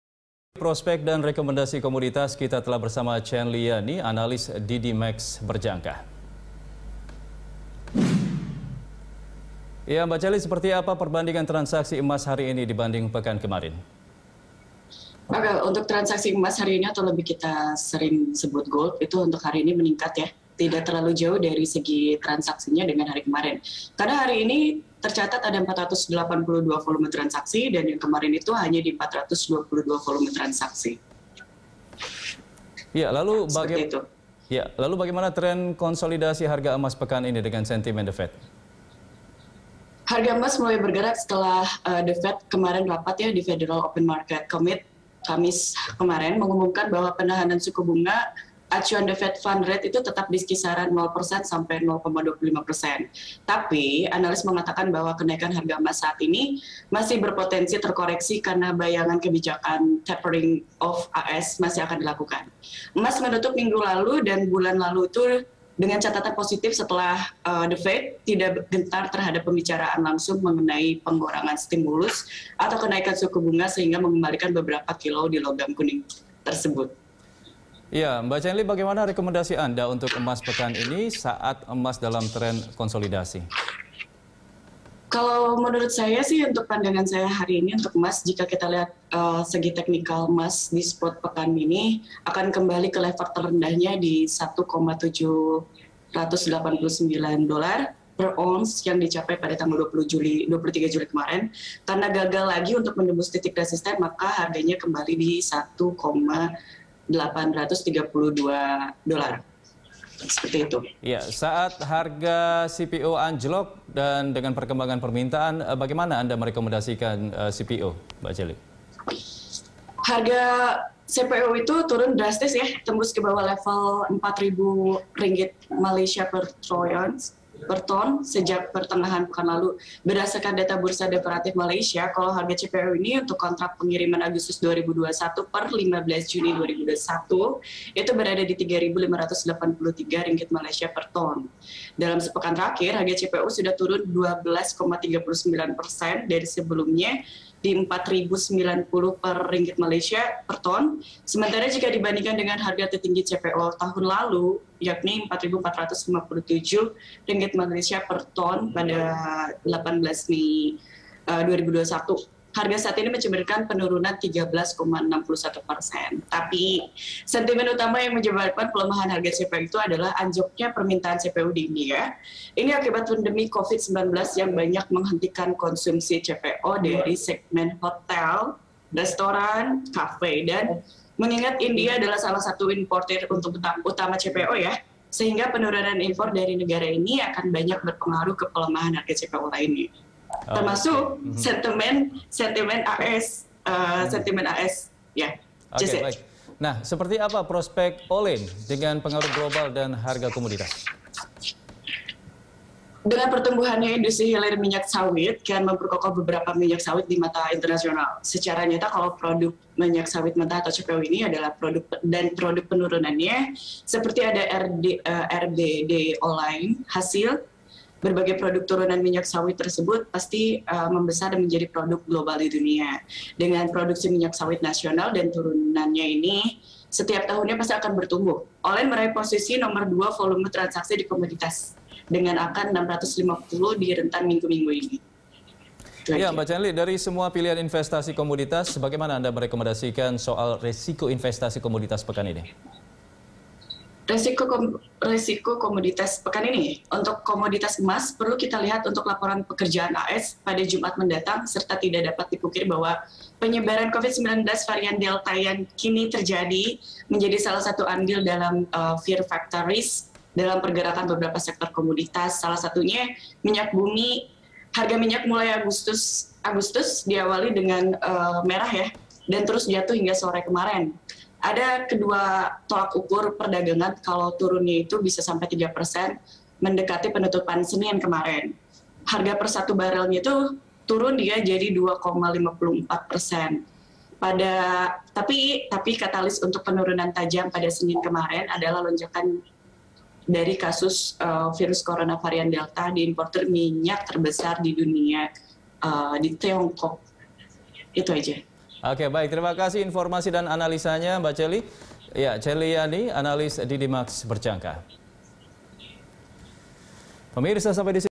Komoditas Emas Meningkat 60 Volume Transaksi
Transaksi emas meningkat tidak terlalu jauh dari segi transaksinya sejak kemarin. "Hari ini tercatat ada 482 volume transaksi dan kemarin hanya 422 volume transaksi," kata Cenli Yani, Analis Didimax Berjangka, Selasa (3/8/2021).